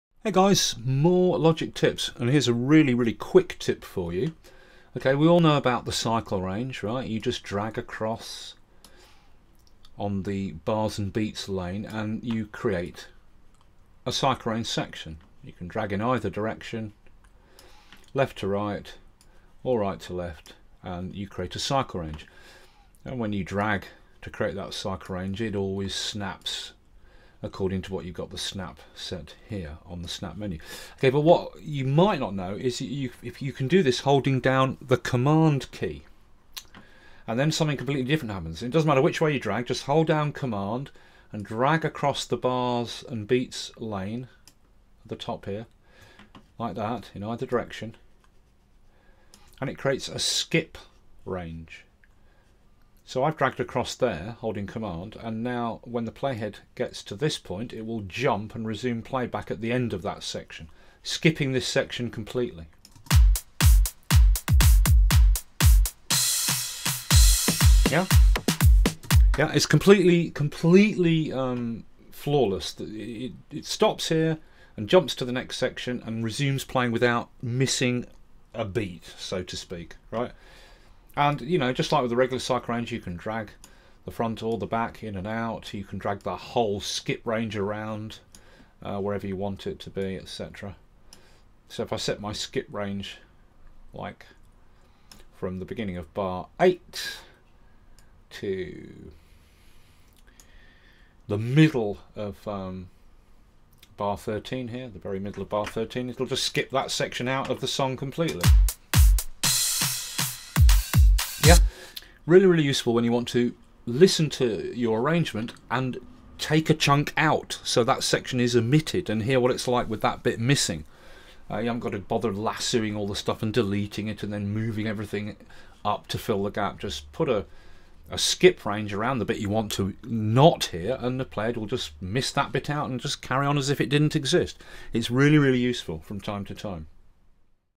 Logic Pro X tips 19 - Skip cycle range
Easily omit any section of your arrangement/song on playback to hear what the arrangement will sound like with any section removed